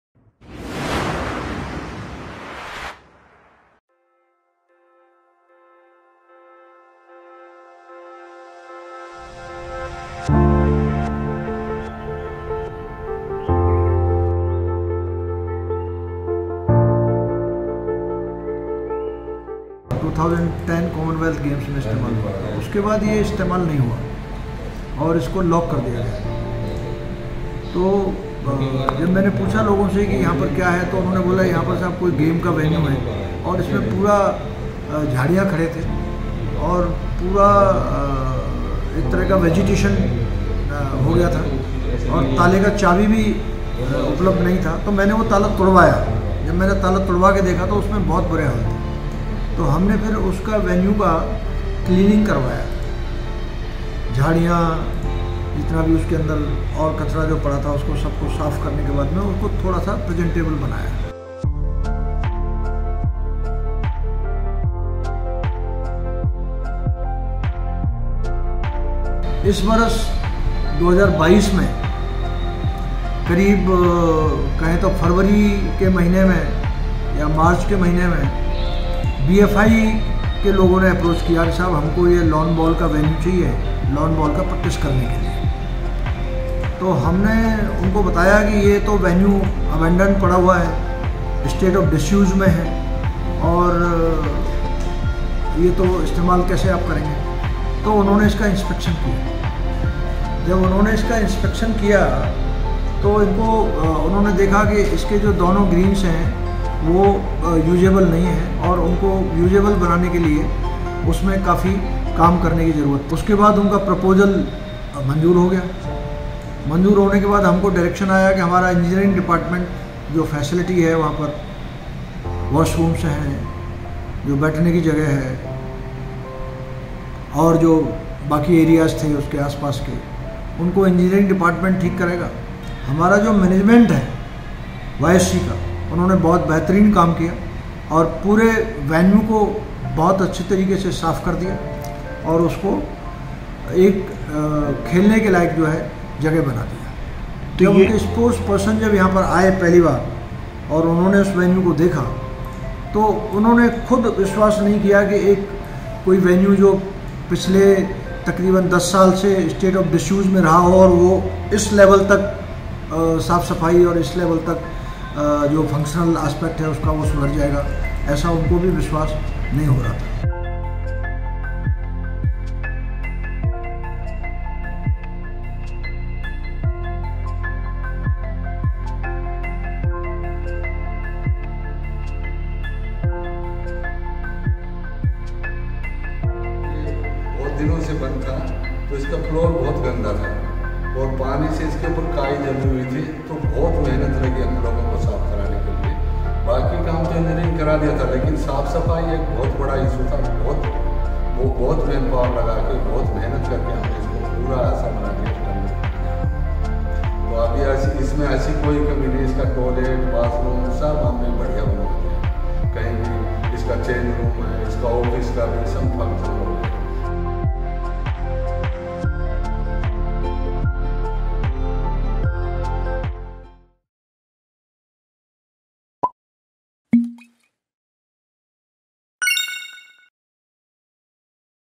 No grounds for Lawn Bowl in Delhi? | Yamuna Sports Complex | The Patriot YSC MM
The international standard Lawn Bowls facility at the Yamuna Sports Complex (YSC) was established for the 2010 Commonwealth Games in New Delhi as a practice venue. However it went into disuse, and the second lawn bowl facility in the capital too was not available for practice.

While YSC is open for Lawn Bowl practice now, there's a possibility it might go into disuse soon as the venue is being allotted to other sports for practice.

Video: Faisal Malik